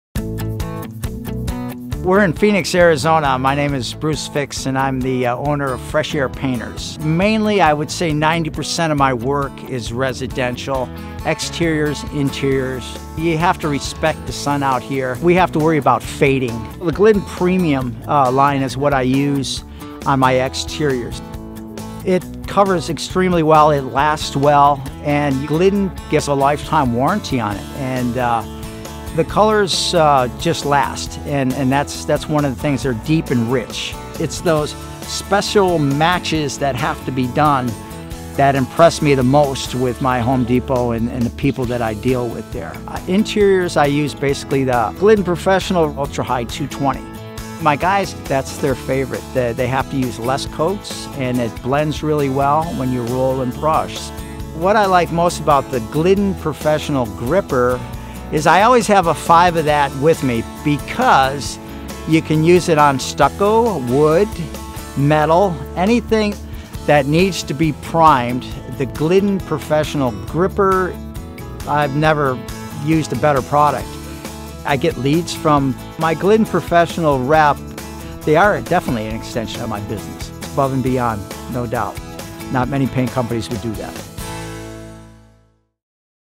Glidden Professional® Customer Testimonial: Fresh Air Painters
Bruce Fix, owner of Fresh Air Painters, has been utilizing the Glidden Professional® line of products for decades. Specializing in residential painting, Bruce relies on the lifetime warranty provided with Glidden® Premium Exterior, the substrate performance of Glidden Professional Gripper® and the quality delivered with Glidden Professional Ultra-Hide® 220.